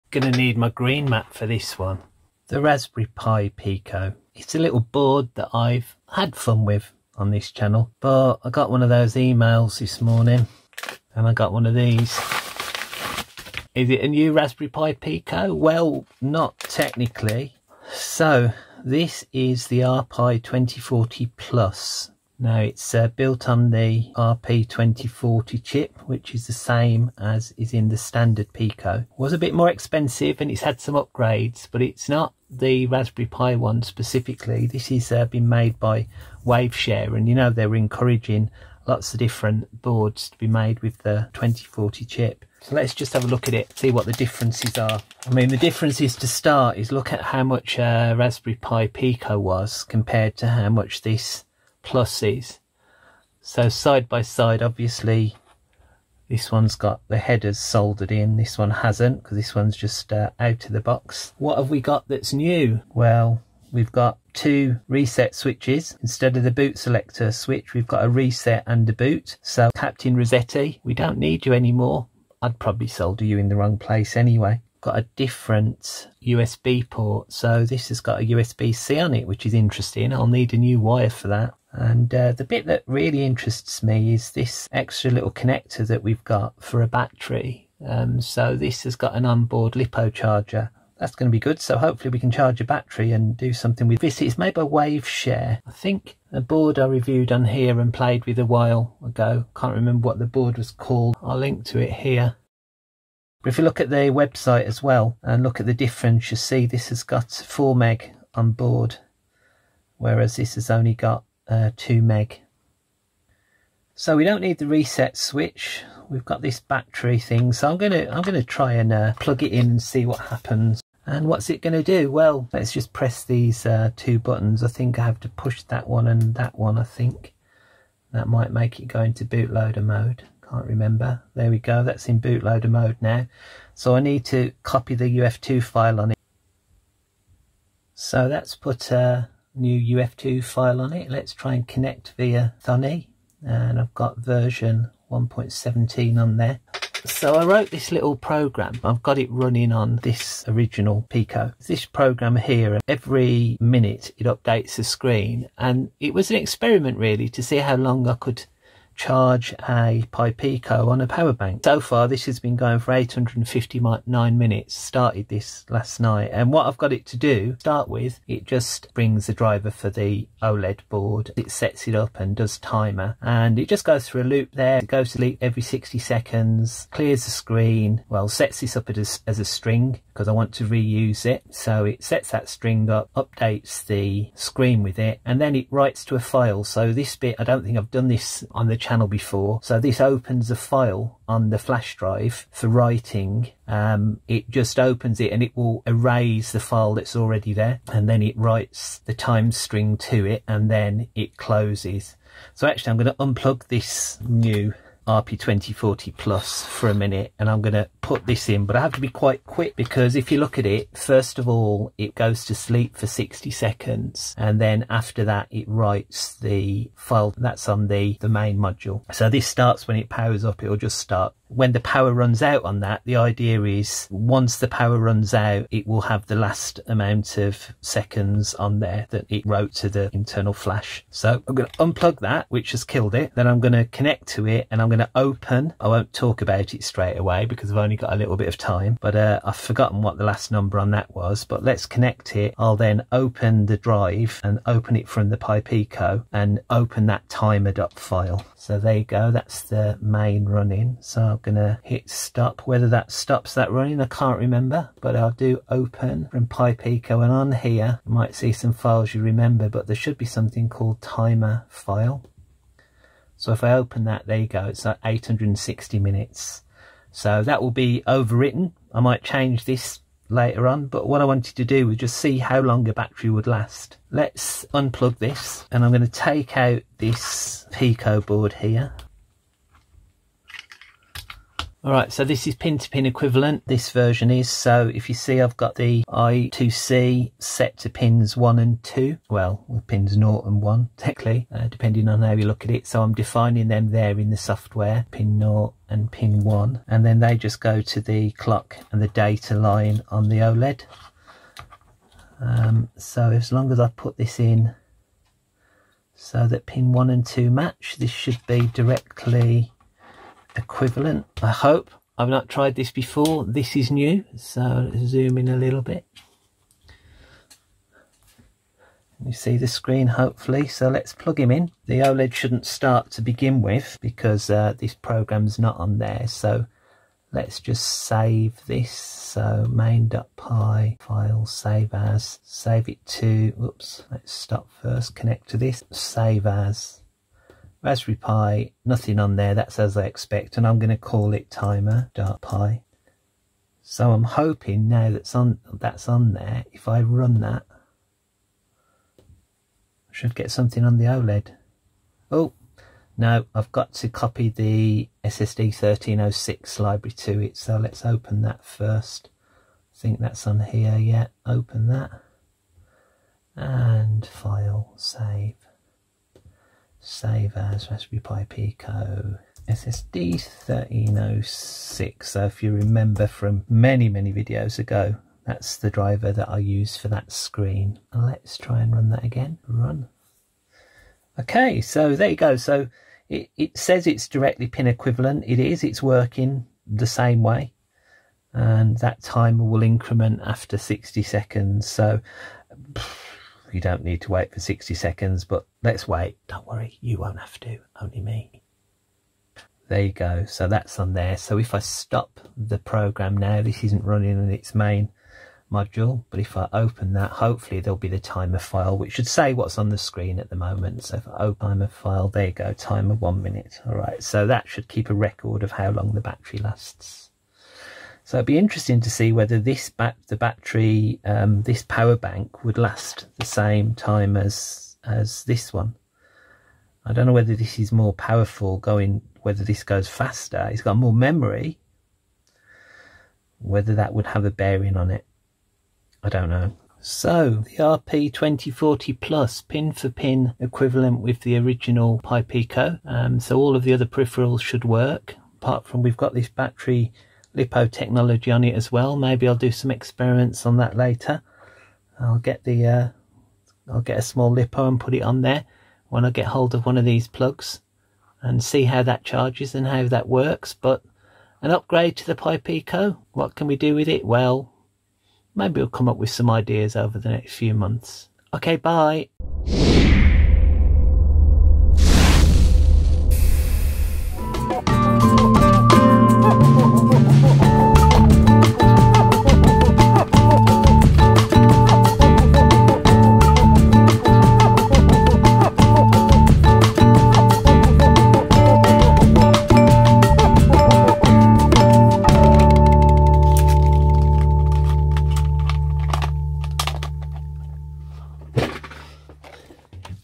RP2040 plus ! A new Pi Pico? First Looks at the Waveshare RP2040 Plus board.
SpiderMaf gets his hands on a new RP2040 based board.  First looks at the RP2040 Plus and comparison with the Raspberry Pi Pico.

Cant call it a box opening.  No Box!